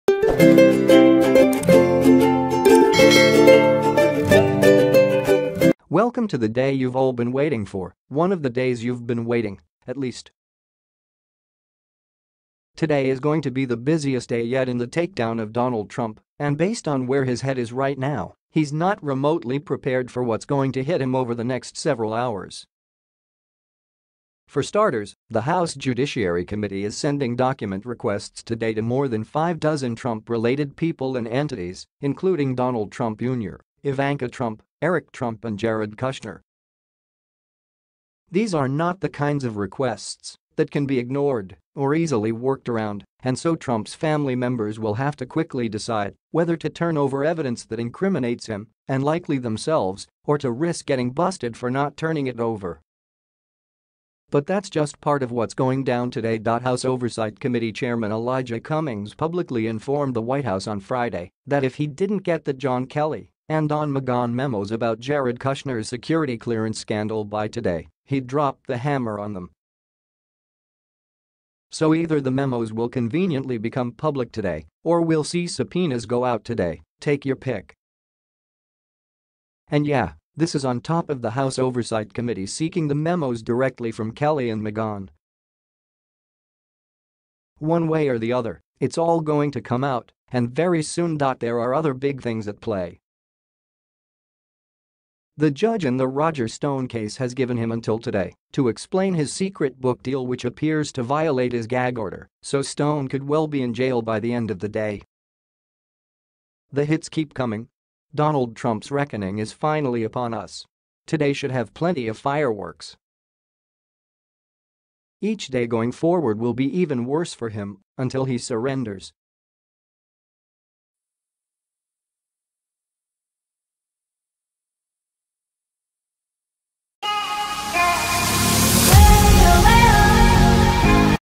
The busiest day yet in the takedown of Donald Trump
Welcome to the day you’ve all been waiting for – one of the days you’ve been waiting, at least.  Today is going to be the busiest day yet in the takedown of Donald Trump, and based on where his head i